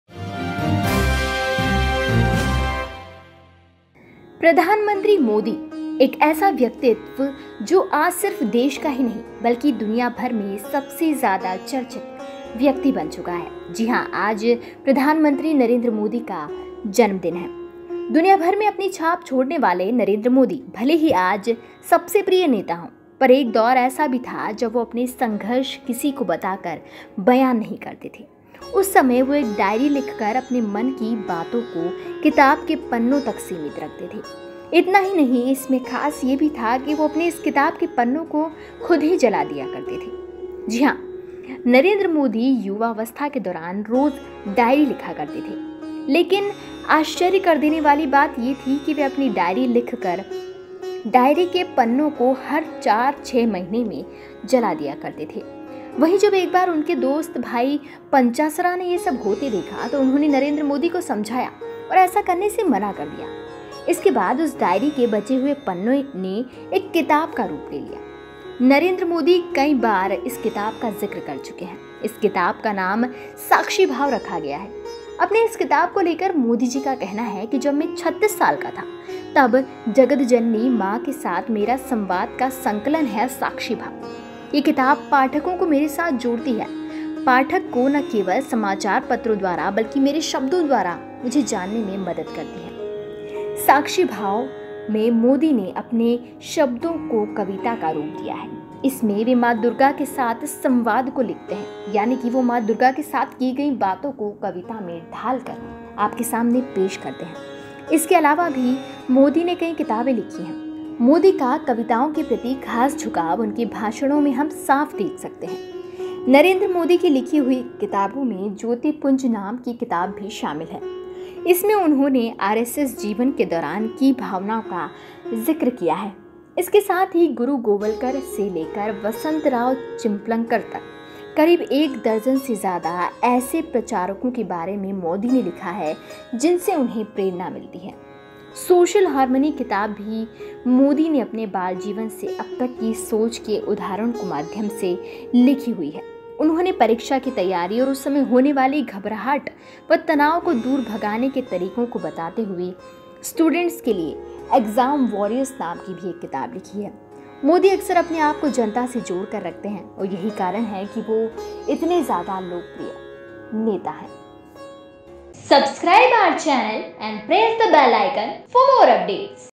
प्रधानमंत्री मोदी की डायरी का राज
जला देते थे जिस डायरी को आज बन गई किताब